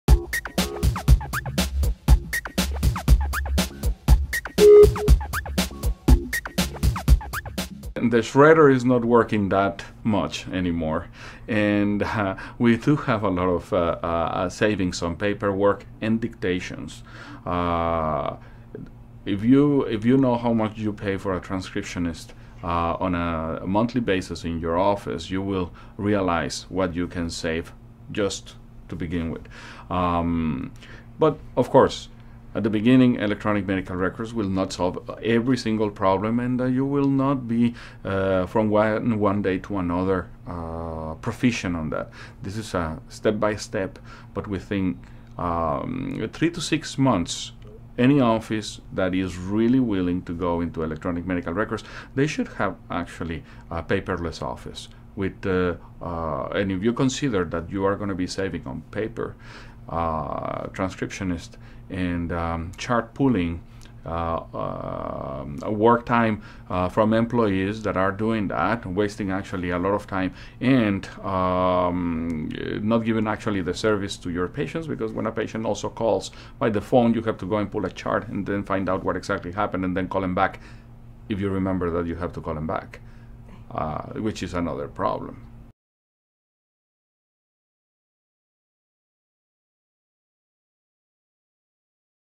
Stories from our clients: Dr Carlos Garcia-Cantú on Cerner Ambulatory EHR
Once a week, we will be sharing a short video from one of our clients. Today we present Dr Carlos Garcia-Cantú discussing Cerner Ambulatory EHR.